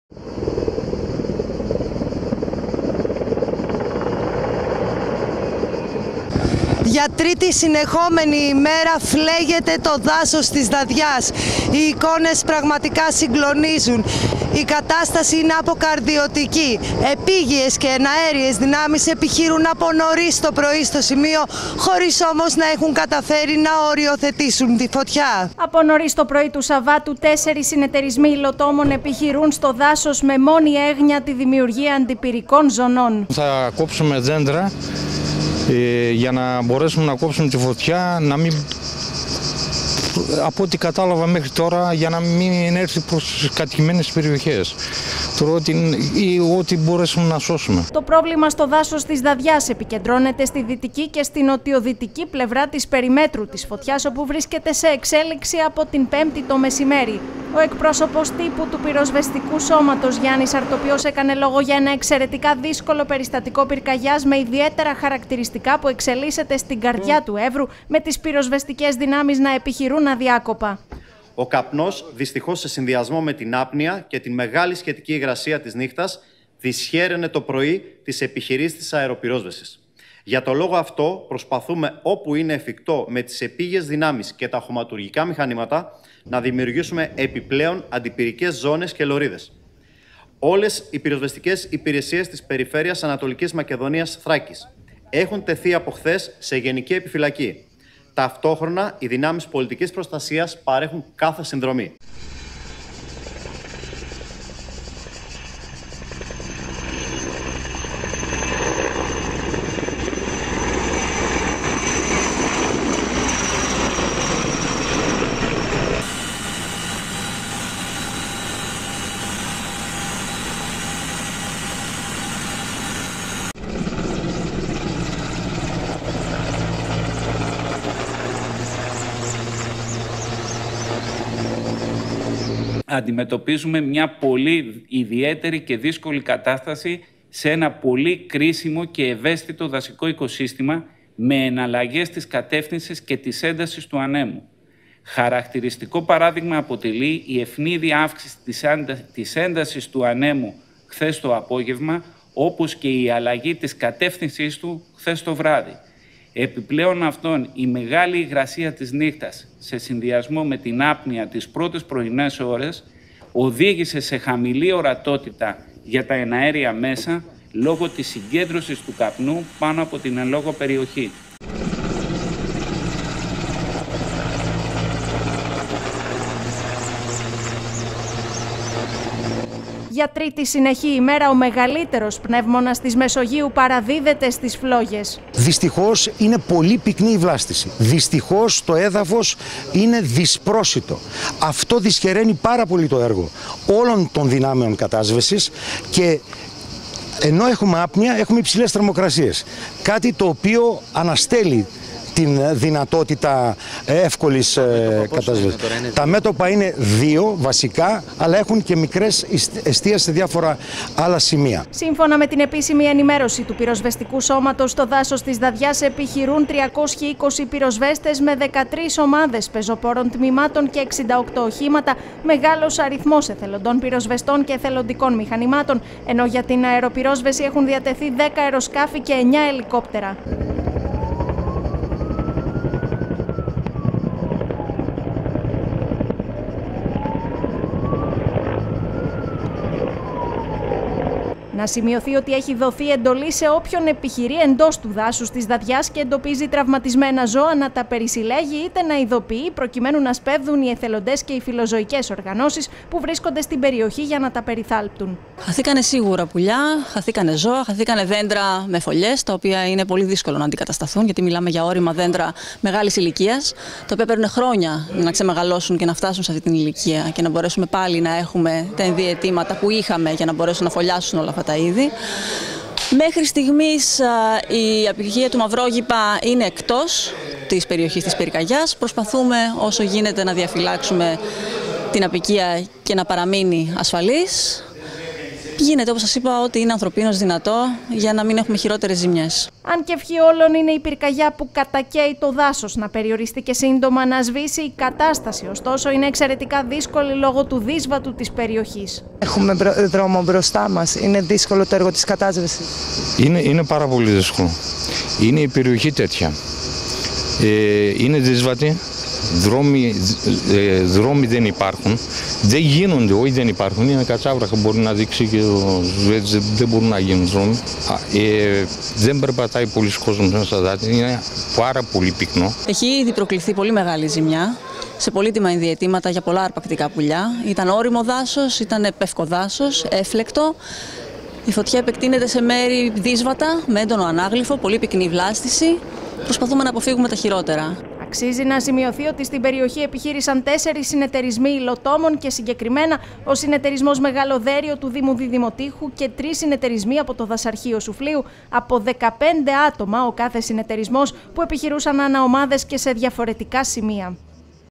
Για τρίτη ημέρα συνεχίζεται η μάχη με τις φλόγες στο δάσος της Δαδιάς
Για τρίτη συνεχόμενη ημέρα η πυρκαγιά μαίνεται στη Δαδιά και κατακαίει χιλιάδες στρέμματα ενός από τα σημαντικότερα οικοσυστήματα της Μεσογείου. Το δάσος της Δαδιάς, που έχει παραδοθεί στις φλόγες, επιχειρούν να σώσουν επίγεια και εναέρια μέσα, πυροσβέστες εθελοντές, ενώ στη μάχη βρίσκονται μηχανήματα του στρατού, των Δήμων και της Περιφέρειας. Από το πρωί υλοτόμοι πασχίζουν κόβοντας κορμούς δένδρων να φτιάξουν αντιπυρικές ζώνες